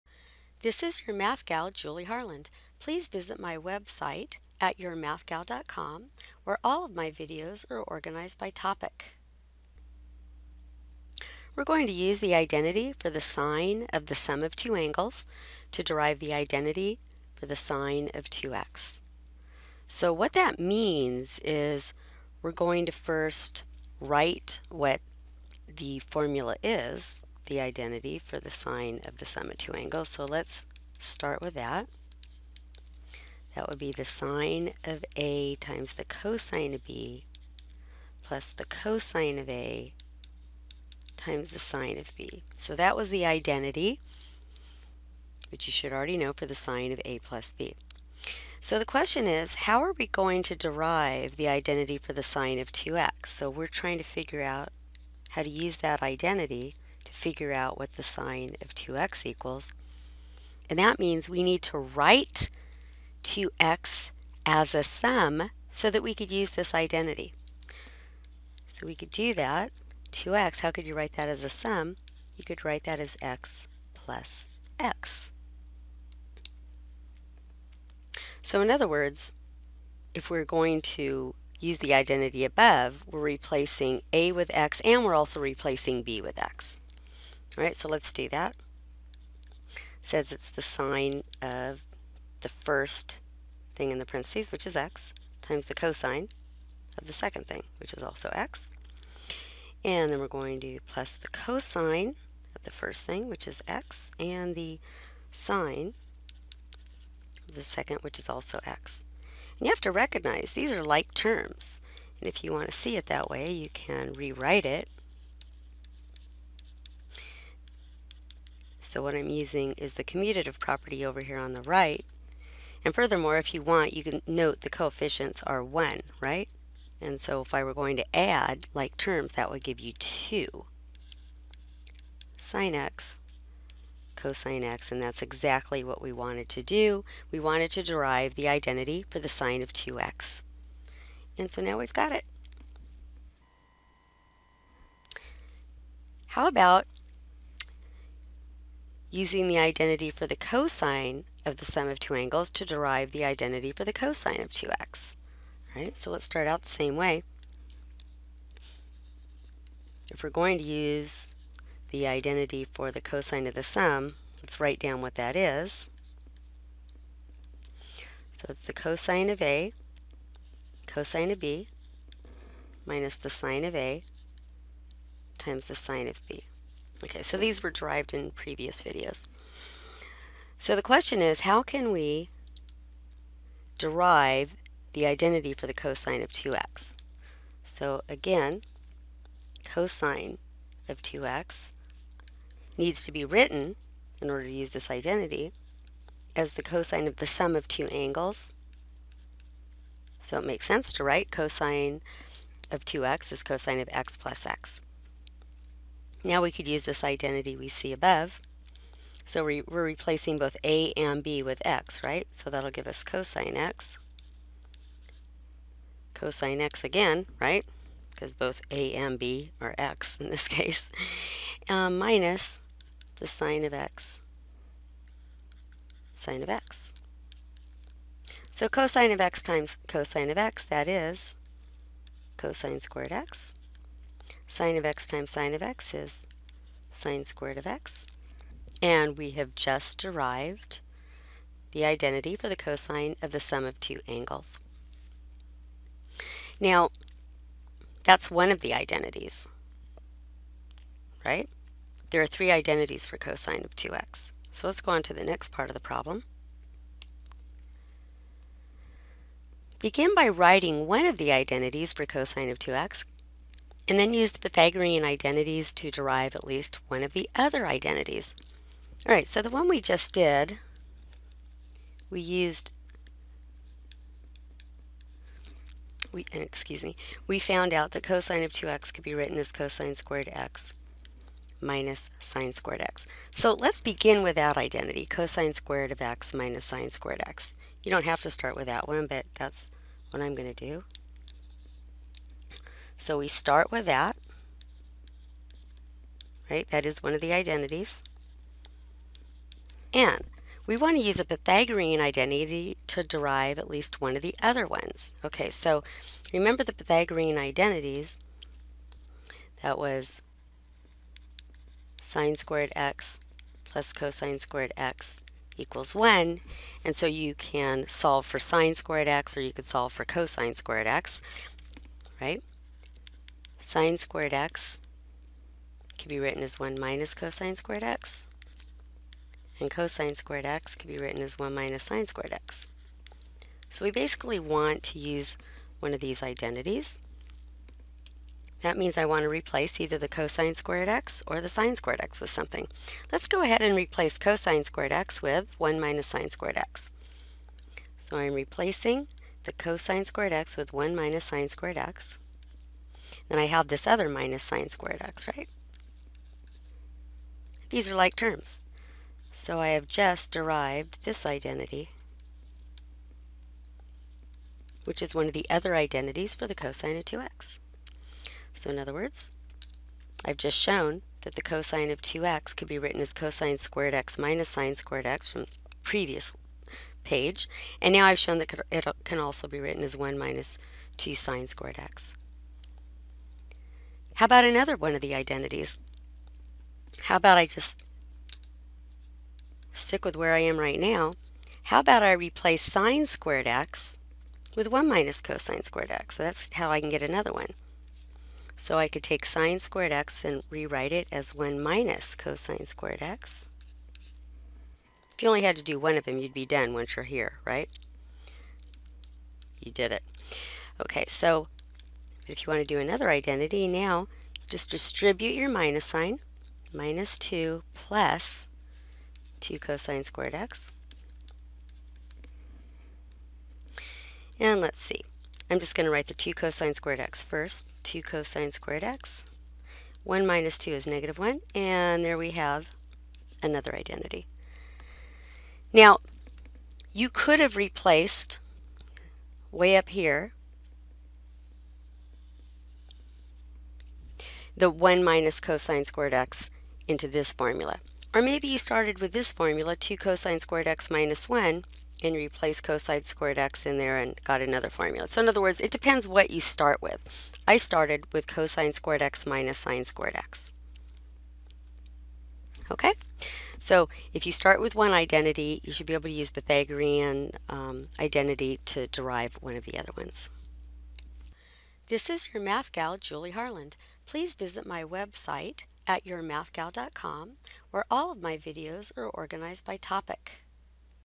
Trig Review 12-14
Part of a set of videos showing solutions to review problems for a Trig Final. Videos by Julie Harland organized at YourMathGal.com.